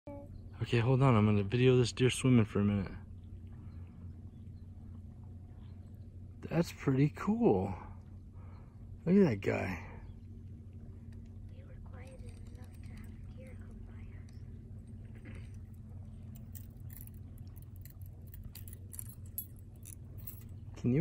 Deer Swimming
Swimming Deer!?
Our friend Neil captured this video of a deer swimming while he was out fishing at Bartlett Lake last weekend.
We were kinda surprised at first, but our biologists tell us deer are actually excellent swimmers! In fact, they can swim up to 13 miles per hour 😱
Fun video, thanks Neil! 🏊‍♂️🦌🙌